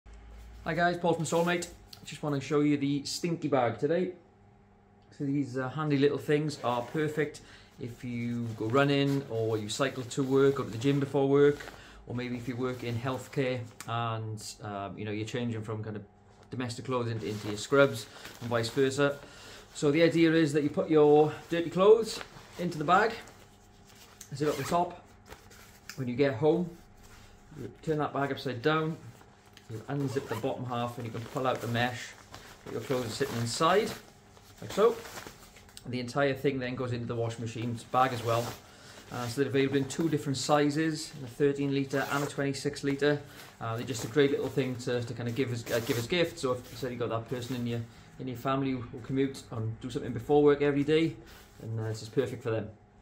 STNKY BAG - Great for sweaty, smelly clothes!
STNKY Bags are the best way to sort, store, carry, wash and dry everything from sweaty gym clothes, laundry when you travel, scrubs, and just about anything else that gets dirty or sweaty.

STNKY Bags lock in those germs and smell from your gear while you go about your day, then, when it's laundry time, there's no need to touch your dirty stuff

Just open the bottom zipper, turn the bag inside out and your STNKY Bag becomes a wash bag

Plastic bags keep in the sweat and the stink, but they're awful for the environment that we love spending time in.  That's why we developed the STNKY Bag. Reusable to minimize environmental damage. Compact enough to fit in a backpack, small suitcase or briefcase. Locks in germs, odors & moisture. And super easy to wash.

Throw it in the washer and dryer, bag and all, and you'll be ready to go again!
Got dirty clothes? Grab a STNKY Bag!

Examples of what you can fit in the STNKY Standard:

-Dirty shoes, shorts and a shirt
-Two pairs of scrubs
-Gym gear and a towel

Examples of what you can fit in the STNKY XL:

- A long weekend trip's worth of clothes
- Four or five pairs of scrubs
- Gear for team sports